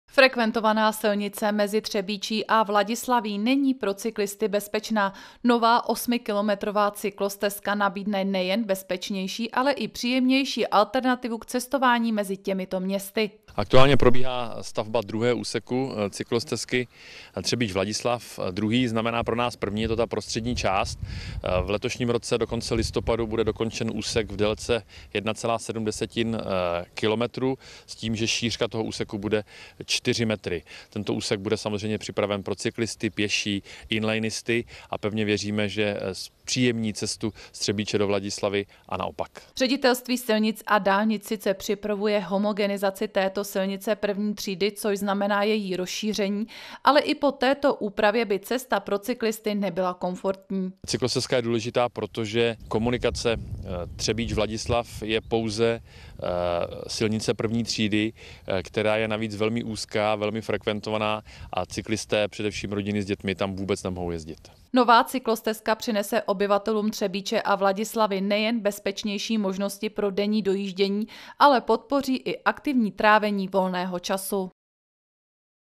Cyklostezka Třebíč – Vladislav
Frekventovaná silnice mezi Třebíčí a Vladislaví není pro cyklisty bezpečná, nová osmikilometrová cyklostezka nabídne nejen bezpečnější, ale i příjemnější alternativu k cestování mezi těmito městy.